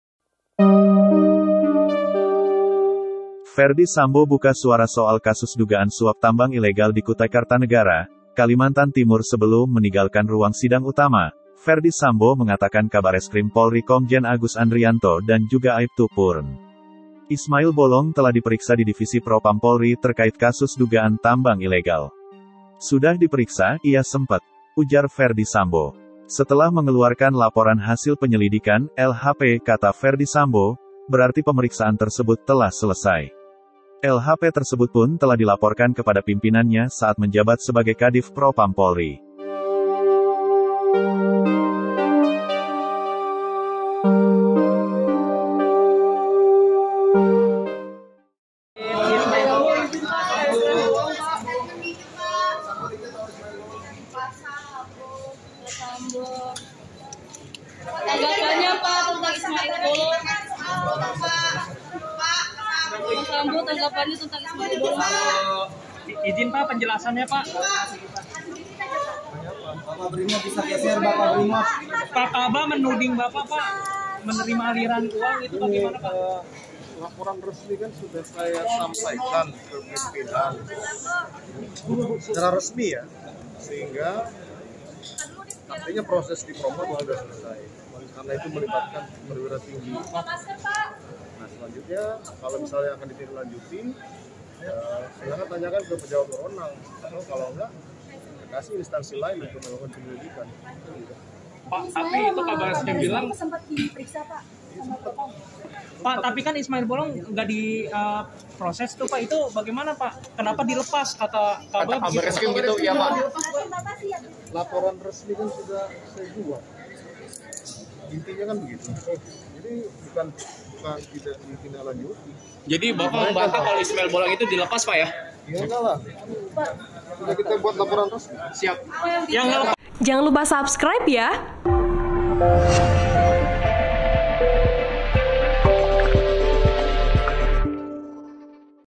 Sambo Sebut Proses Tambang Ilegal di Propam Sudah Selesai Pemeriksaan
WARTAKOTALIVE.COM, PASAR MINGGU- Ferdy Sambo buka suara soal kasus dugaan suap tambang ilegal di Kutai Kartanegara, Kalimantan Timur

Sebelum meniggalkan ruang sidang utama, Ferdy Sambo mengatakan Kabareskrim Polri Komjen Agus Andrianto dan juga Aiptu (purn) Ismail Bolong telah diperiksa di Divisi Propam Polri terkait kasus dugaan tambang ilegal.